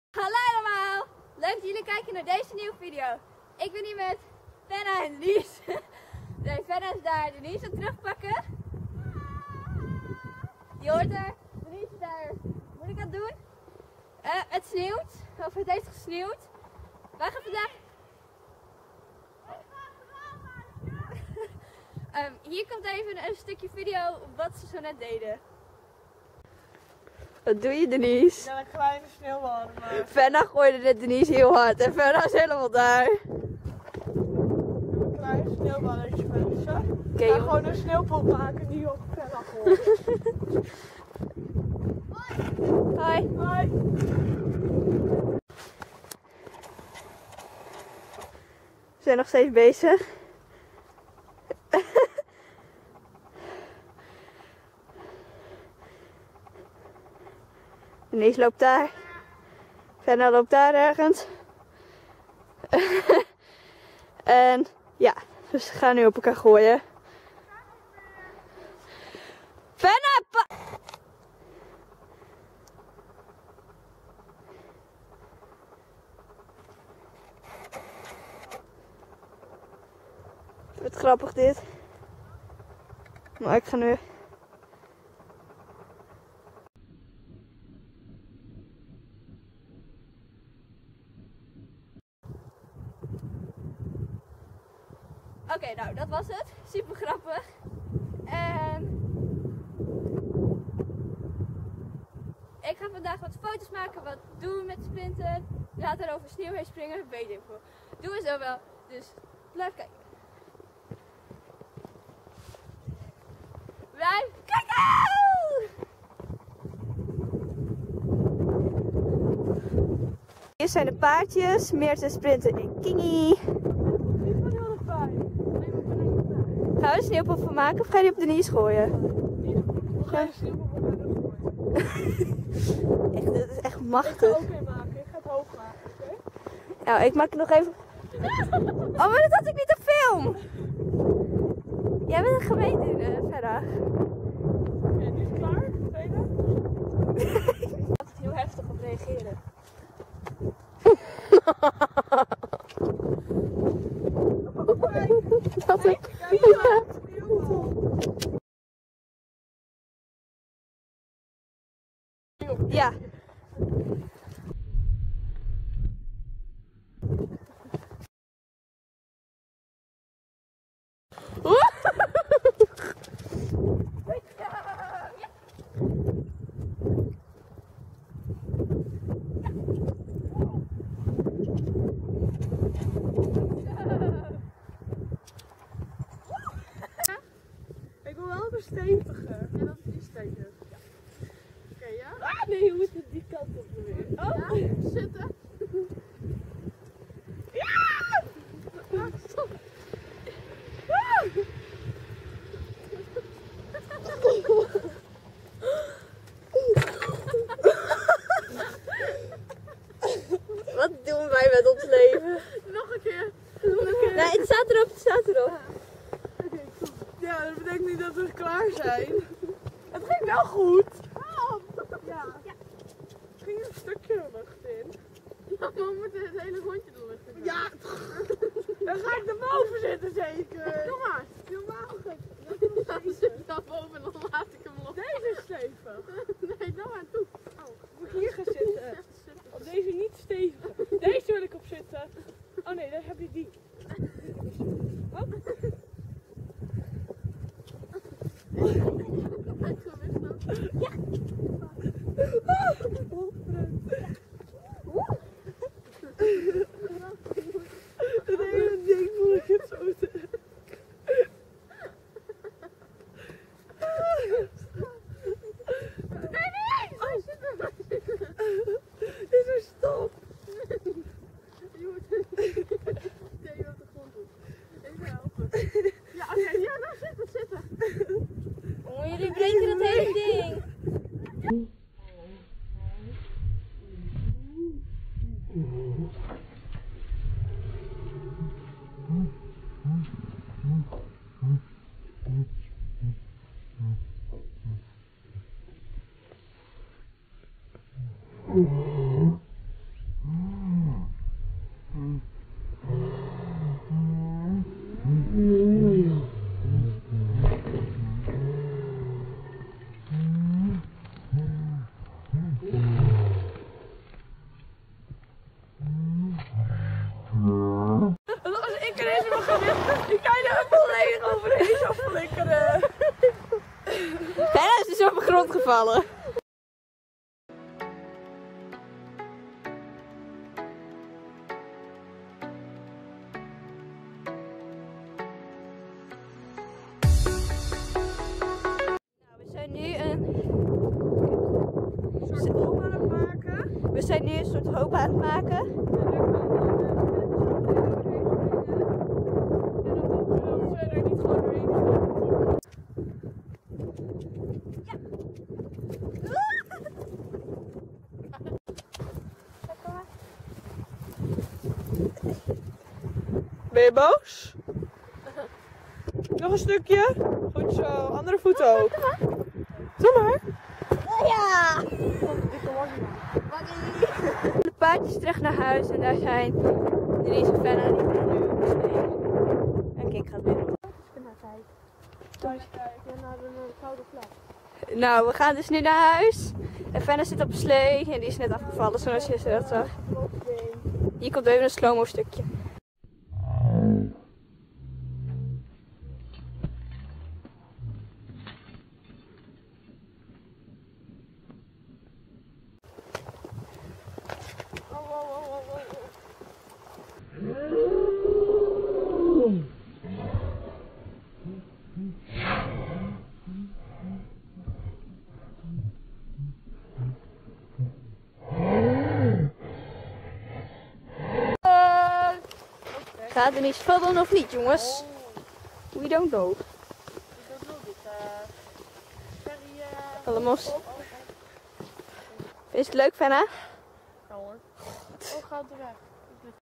TRAMPOLINE BREEKT & OVER SNEEUW SPRINGEN
ik ging met fenna en denise in de sneeuw op pad
dus we ging kloten